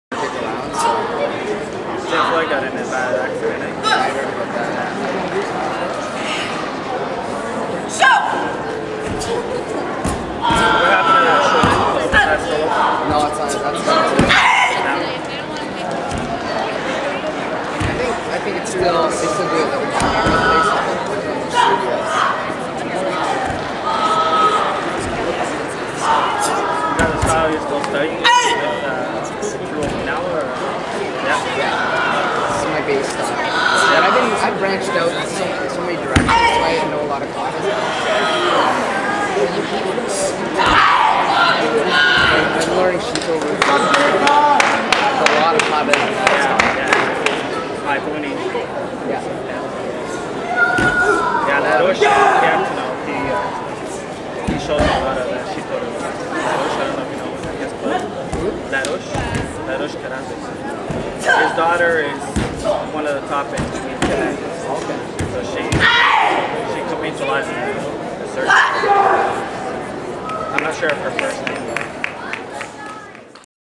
Michelle Maizis - 2007 Canadian Open Kata
Michelle Maizis's Kata performed at the 2007 Canadian Open Martial Arts Championships in Hull, Quebec Canada